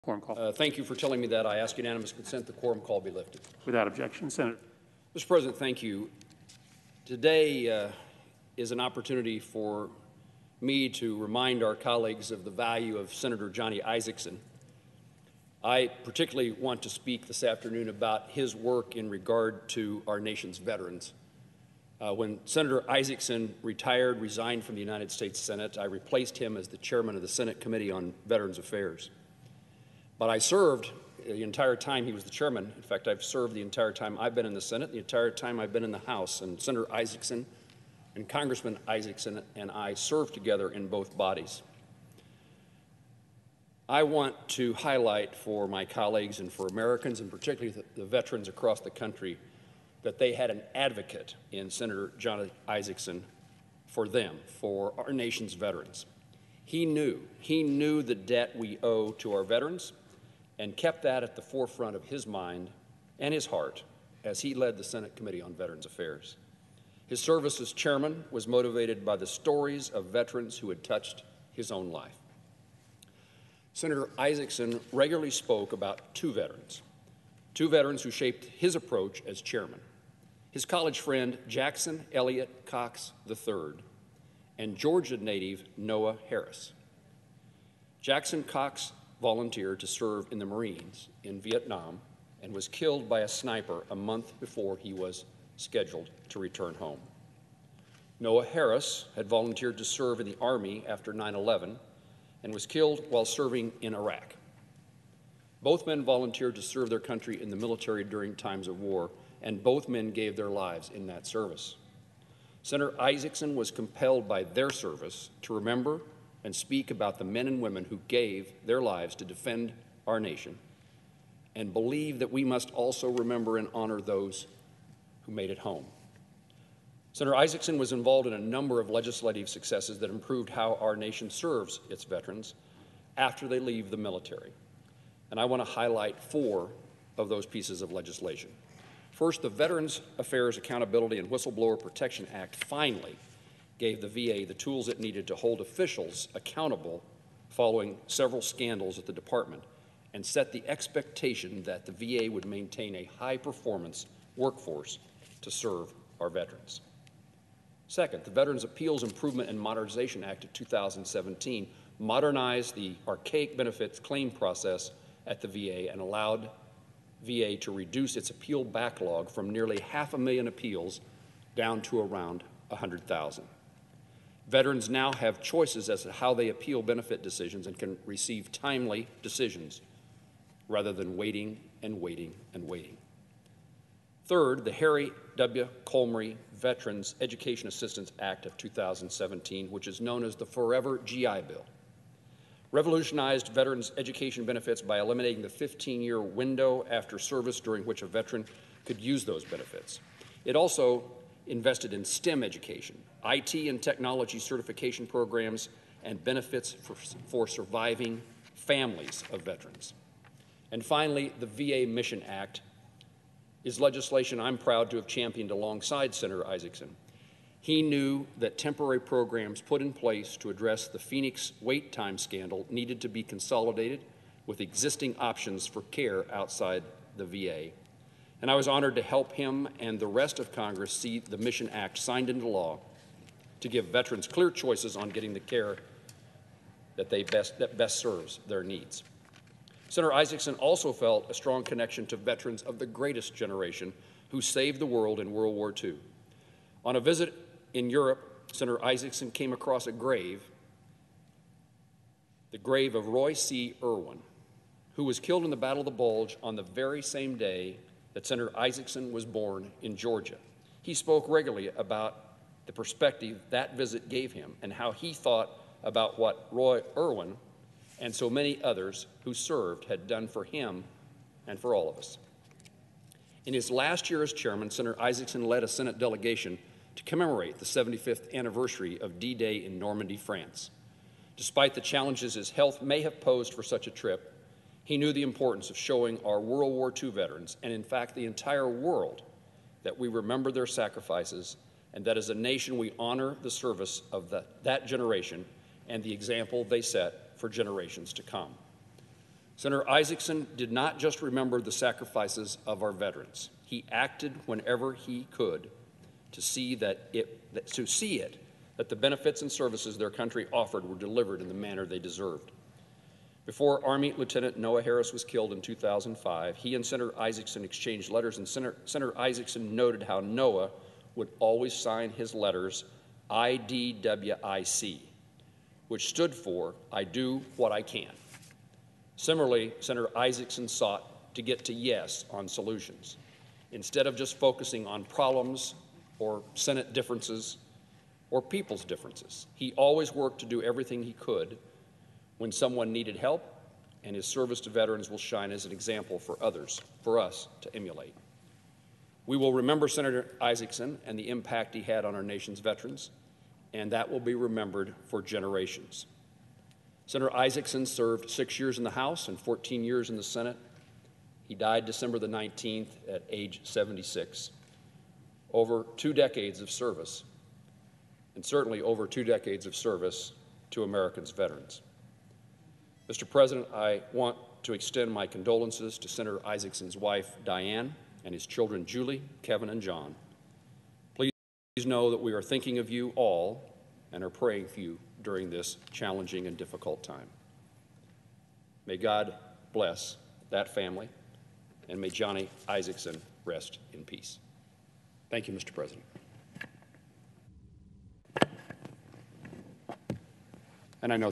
GOP Senator Pays Tribute To Johnny Isakson
Sen. Jerry Moran (R-KS) took to the Senate floor on Tuesday to pay tribute to the late Johnny Isakson.